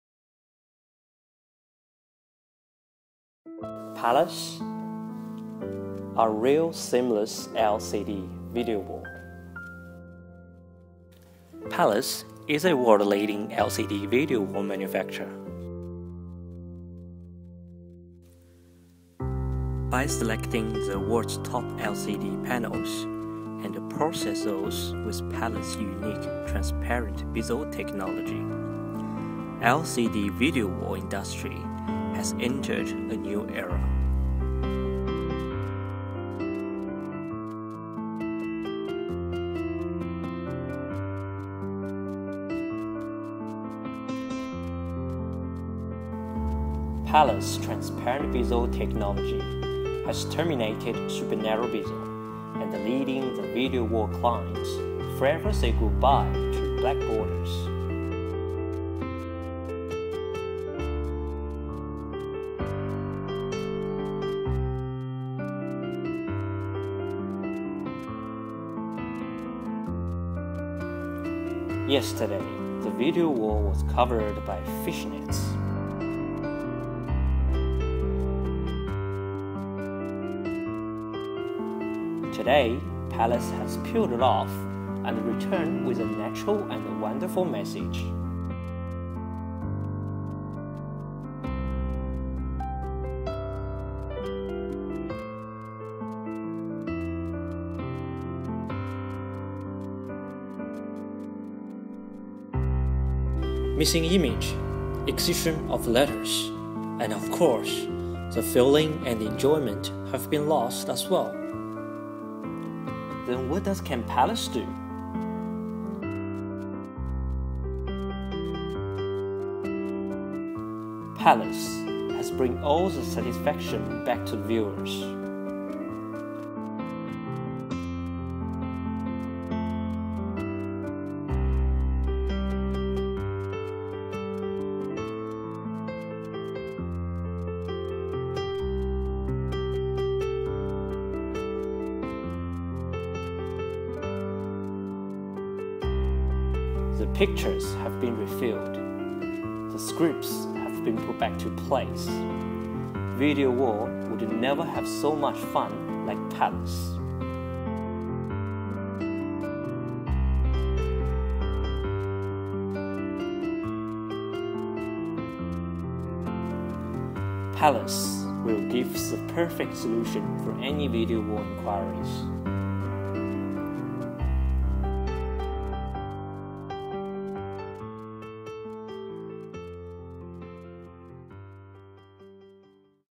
Pallas HD
Pallas is introducing the latest technology of 0mm seamless LCD video wall. Transparent bezel makes 0mm seamless not only a dream; 2x2 Multi ultra HD LCD televisions make video wall 4K; 2x3,3x4 even 4x5 makes seamless video wall unlimited expansion. 4K? 8K? We can make Unlimited K!

Pallas, leads the digital industry into a new era.

Welcome to our showroom for immersive sensation.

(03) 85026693
4 Joyce St, Springvale VIC 3171